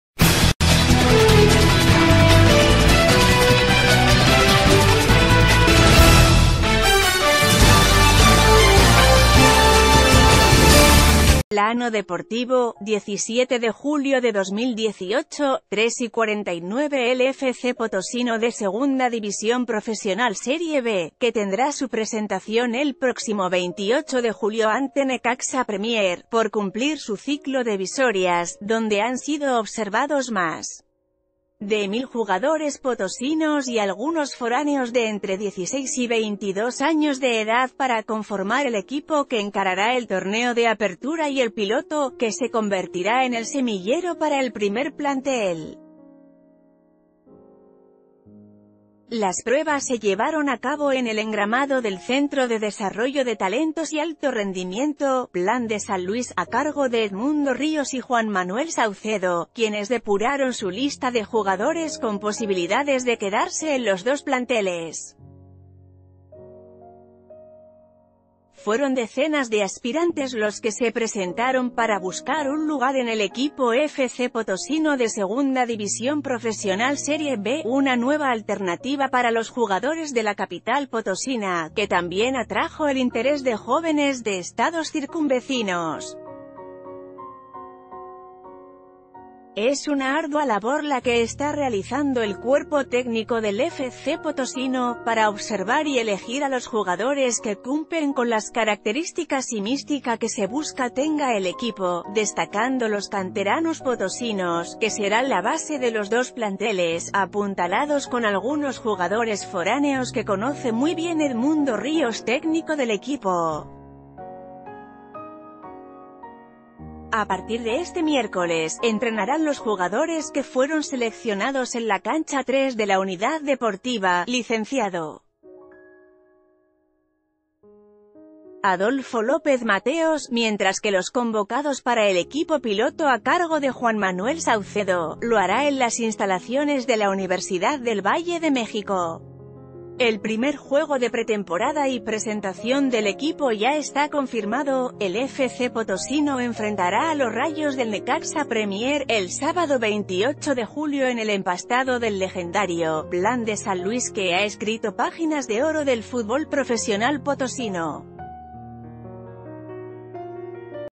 FC Potosino jugará ante Necaxa Premier
Plano Deportivo | 17 Julio 2018 | 15:49 El FC Potosino de Segunda División Profesional Serie B, que tendrá su presentación el próximo 28 de julio ante Necaxa Premier, por cumplir su ciclo de visorias, donde han sido observados más de mil jugadores potosinos y algunos foráneos de entre 16 y 22 años d...